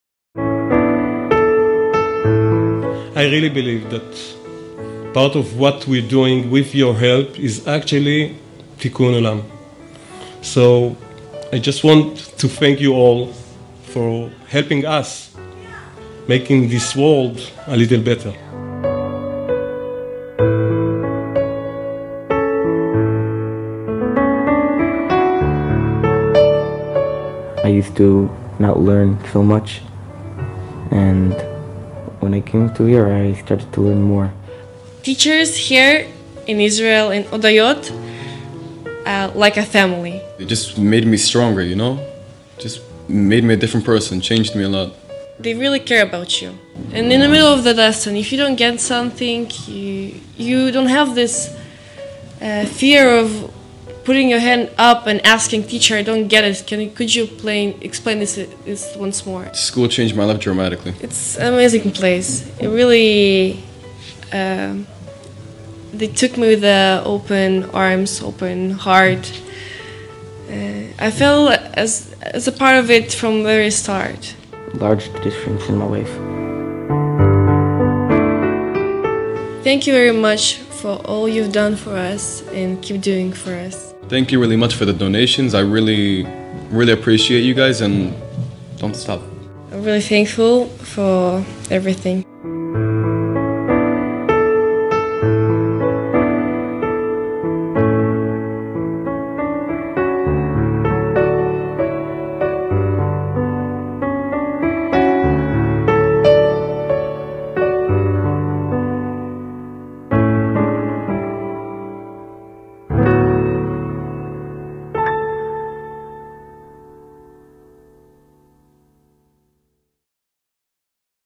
Leaders and students from Hodayot Youth Village
How do you help children find their way into an adult world which has always let them down? A programme to provide horses for Israel’s police force is one solution for students at World ORT’s Hodayot Youth Village.

Children with emotional problems join peers on the school’s unique police studies track to look after and train the horses for three years when they are handed over to the police for active service.

“There are some kids who come here to train the horses and being with them calms them down, gives them a chance to get things out of their heads,” said Avi, pictured here with Sufa (Storm).

The youth village is often the last chance for children from dysfunctional families, children with behavioural or emotional problems, and those who have been expelled from other schools. Most of them are from immigrant families, many from the Ethiopian community, and all are from socio-economically disadvantaged backgrounds.

Produced by video production company 10ten Studios
Shot by Tony Gorkiewicz